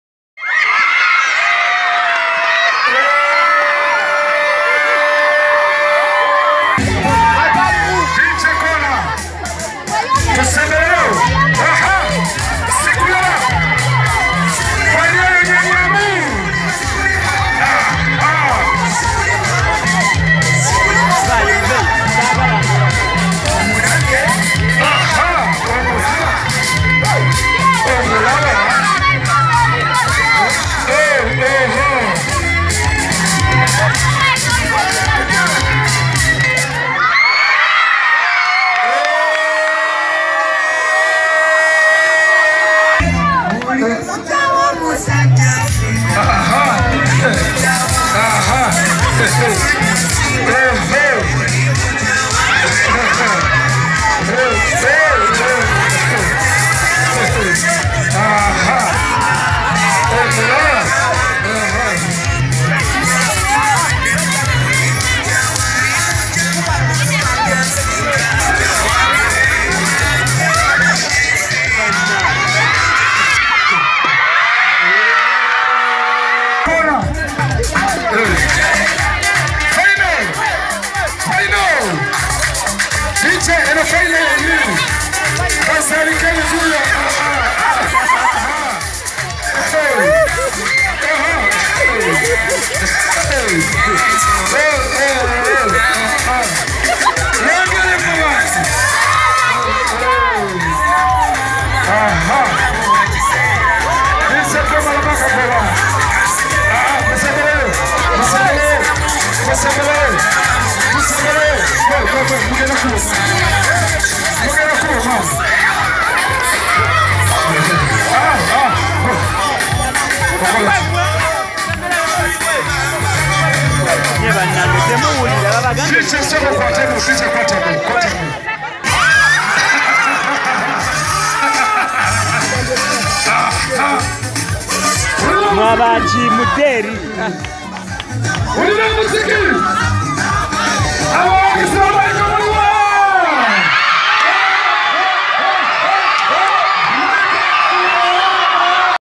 Chair Dance Challenge
The entertainment continues while we were celebrating with primary students from Makindye Division schools .

Young people need to be encouraged to Stay in school.